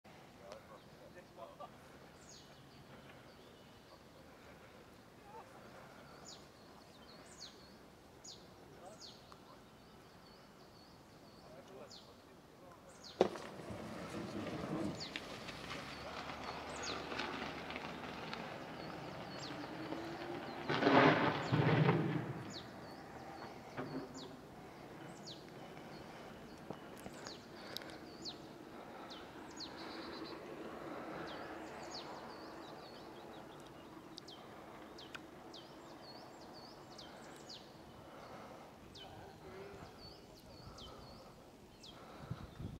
Twin 3 Seilriss [HDC-SD10 Test]
wollte einfach mal meinen neuen comcorder ausprobieren.
Seilrissübung in ca. 70m.

Camcorder : HDC-SD10